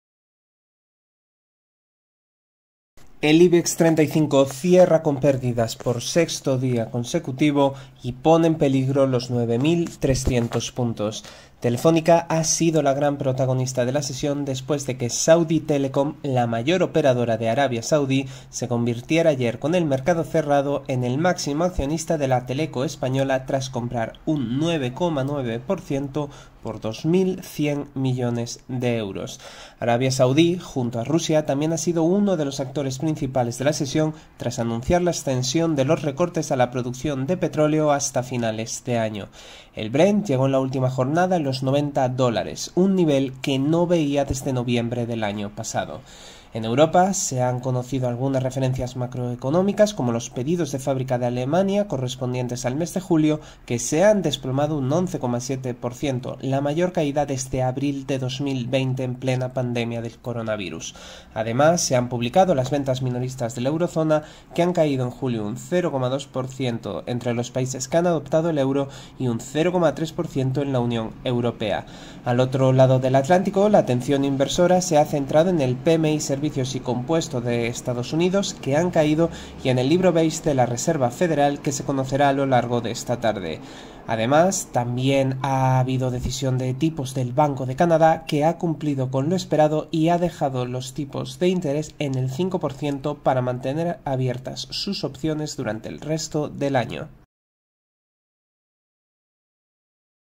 El Ibex 35 cae por sexto día consecutivo tras la operación de Telefónica y Arabia Saudí
Cierre Ibex 35 - 6 septiembre - El Ibex 35 cae por sexto día consecutivo tras la operación de Telefónica y Arabia Saudí